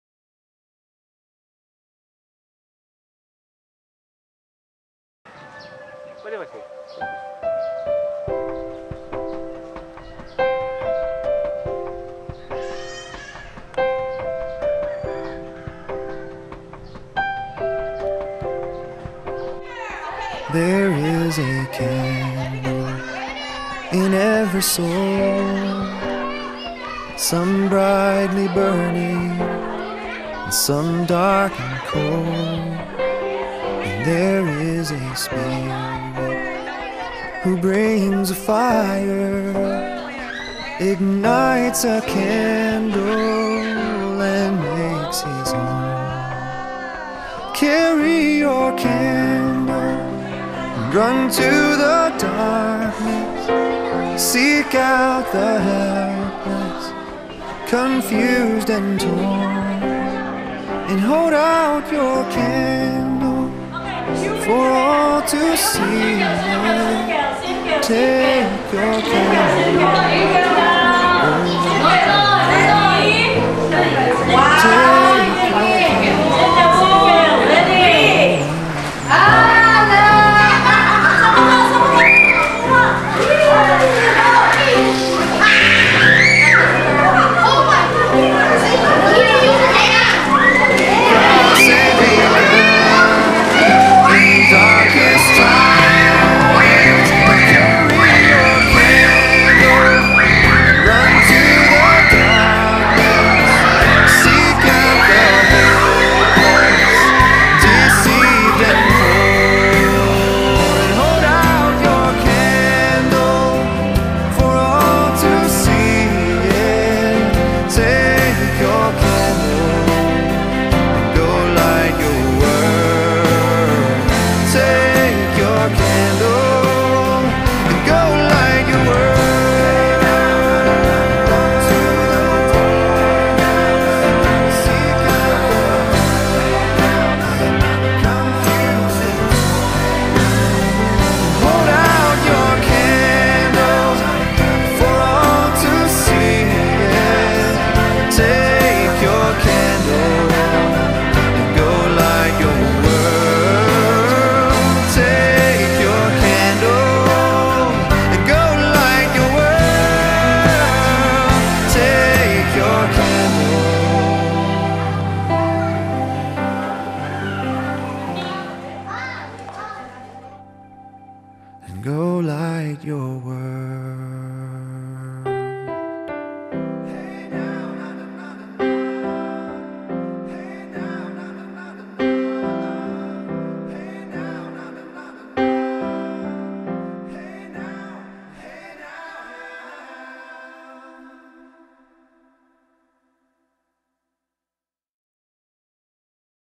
Orphanage Visit 2008 in Zamboanga City, Philippines
Visiting and giving clothes, food, and diapers to the 42 orphaned children at an orphanage in Zamboanga City, Philippines in 2008